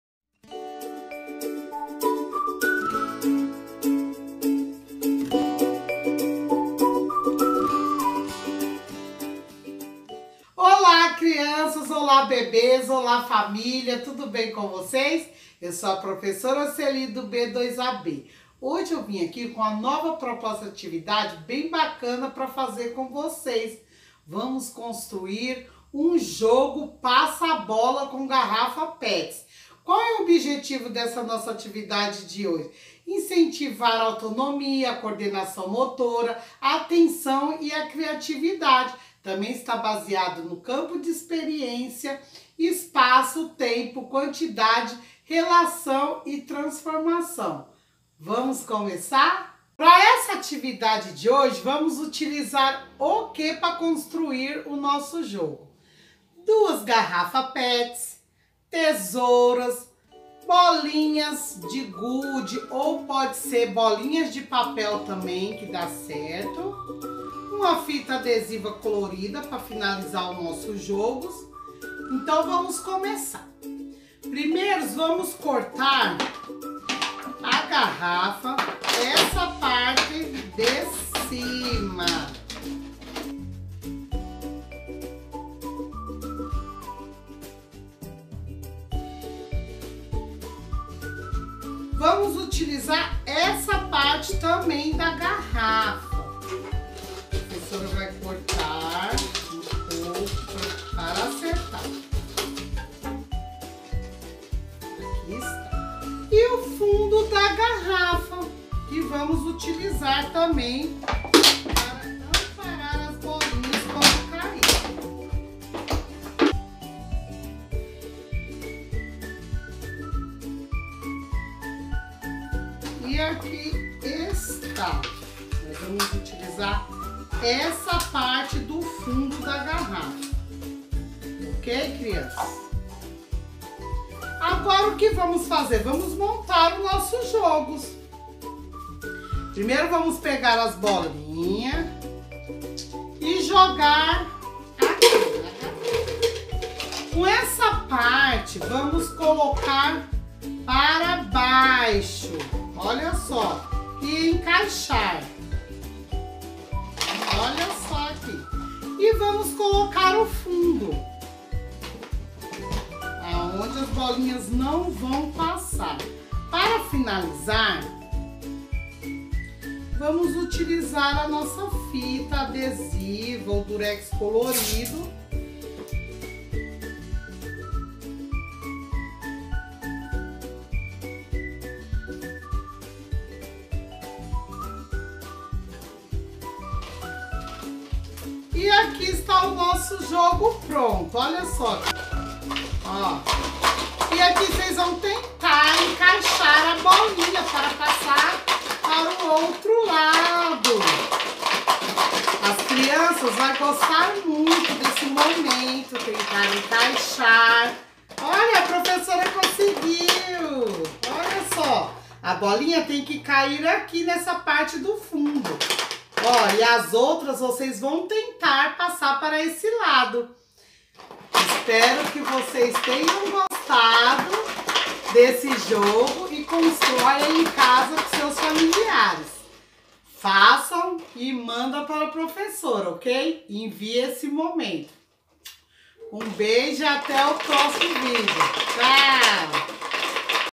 Atividade: Jogo Passa Bola - CEI Jardim Arpoador
Vídeo de caráter educativo, produzido pela professora Celi Lage do CEI Jardim Arpoador para os bebês do Berçário I e II A/B/C/D e para as crianças do Mini grupo I A/B/C/D e Mini grupo II A/B/C como forma de atividade durante o período de isolamento social.